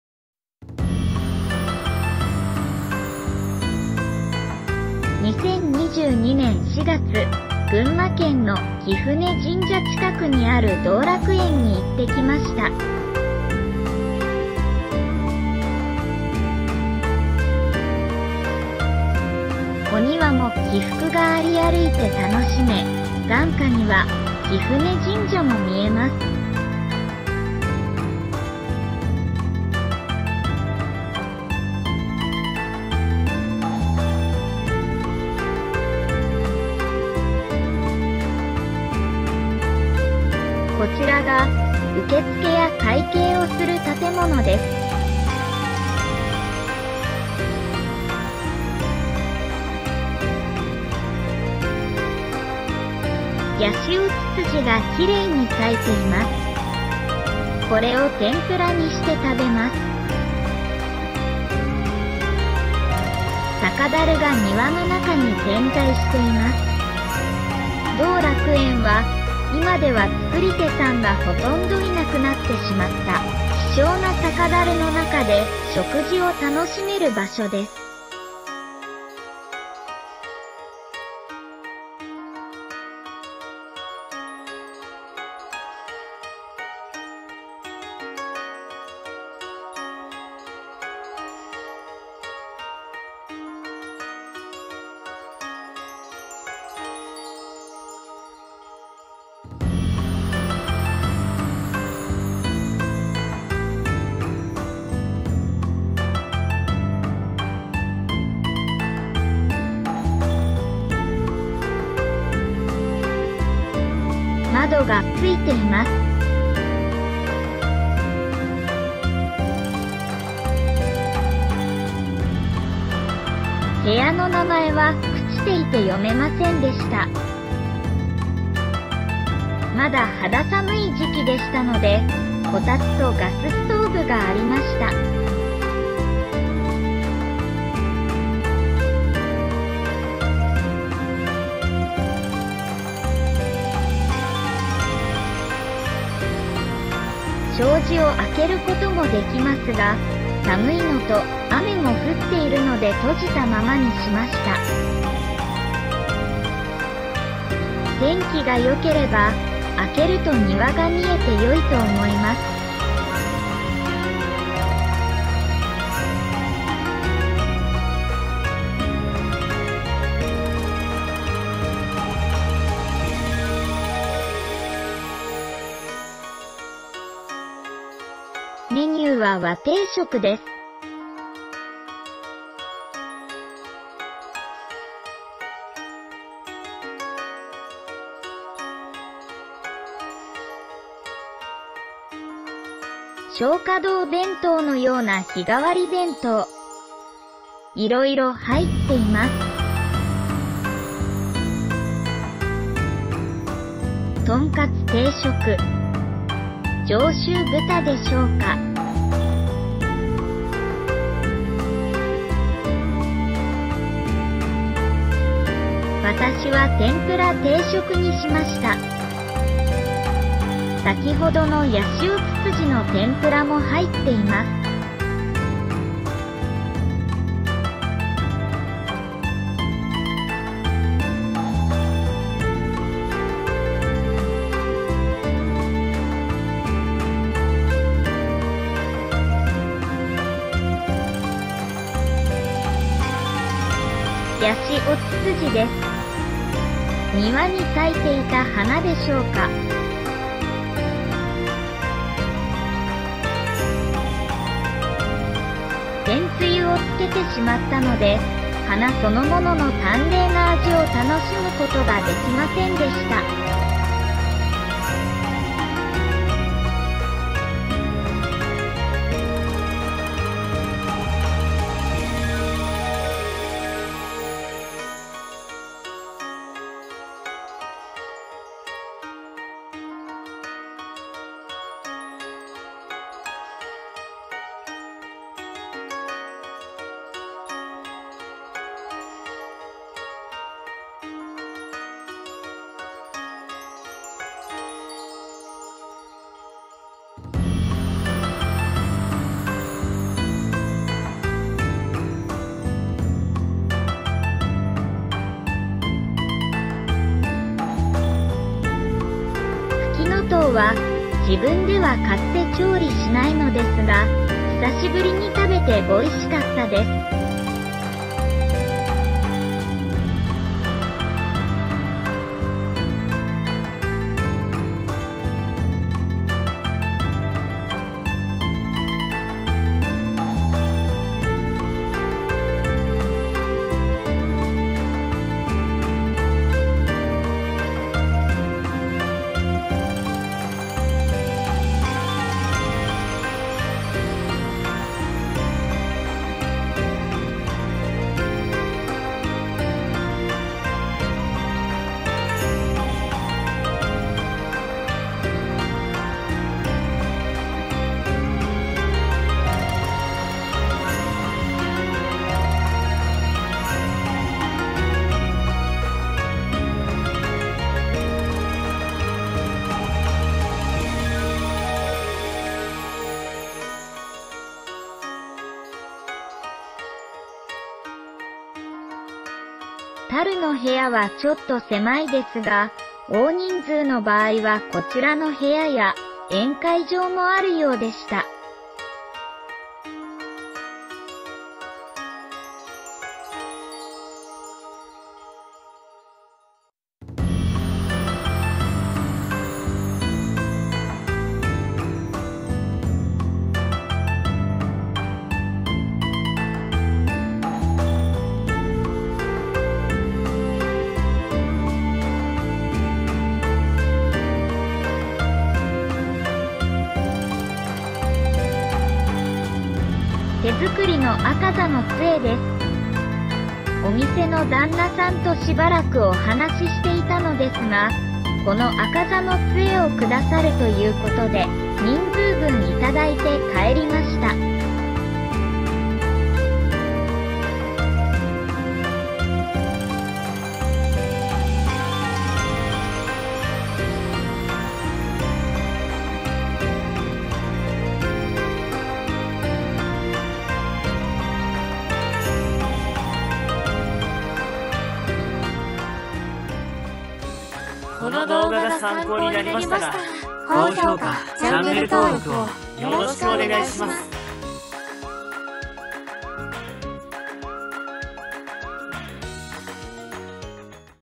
0279【道楽園跡継ぎ募集中】酒樽の中でご飯：グルメ【群馬県みどり市】
群馬県みどり市にある、貴船神社のすぐ近くにある道楽園にランチを食べに行きました。
酒樽の中でのランチです。

2023年、跡継ぎ募集中だそうです。

◆音声：音読さん（ななみ）
速度: (×0.6)
高低: (4)